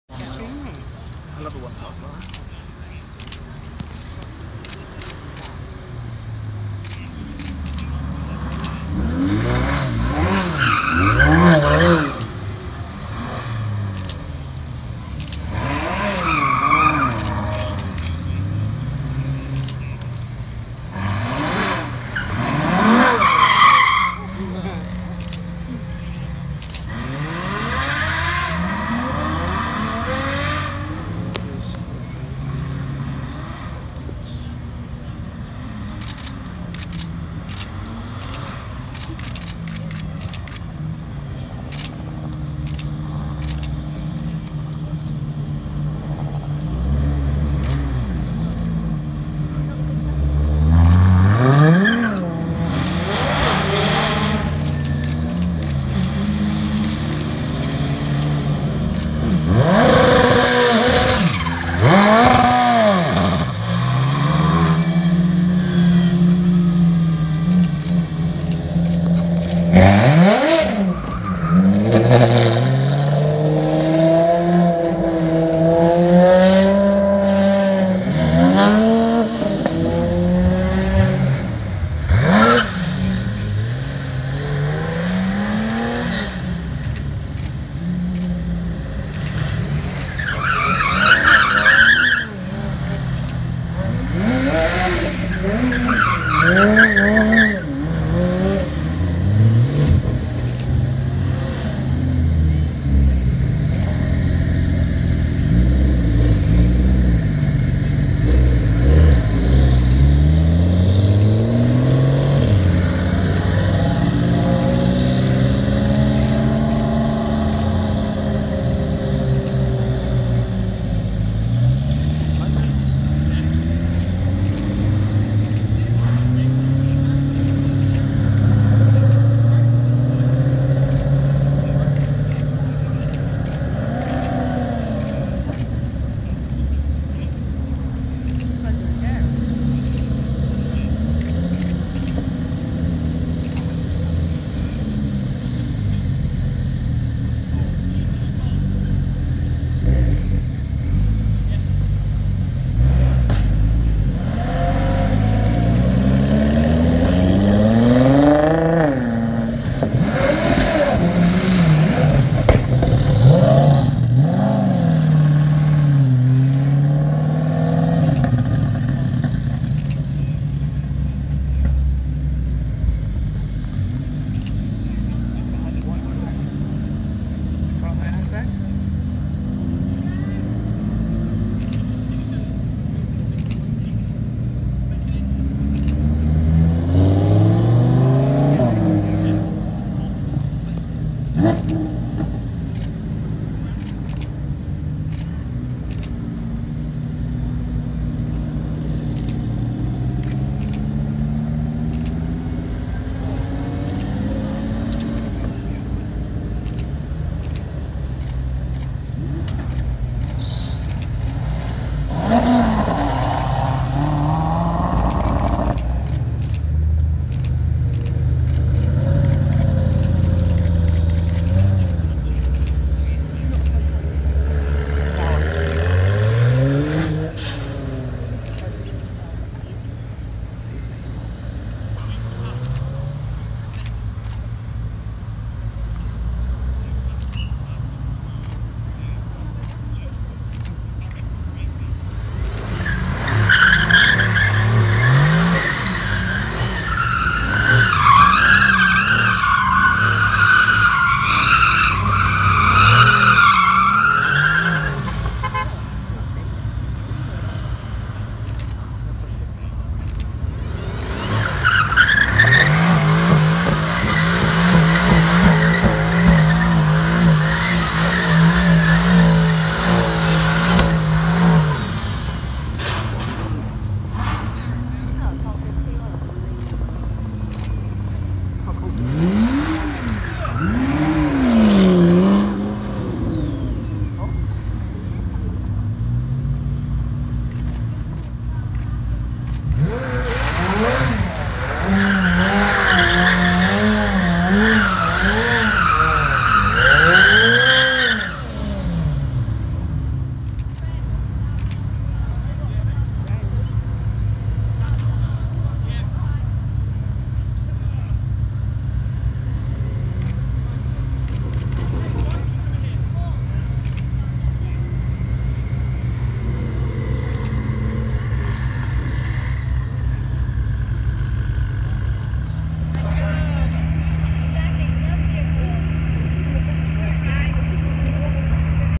Supercars @ Goodwood Festival of Speed Day 2
A bigger selection of the supercar paddock take to the hill in the morning session.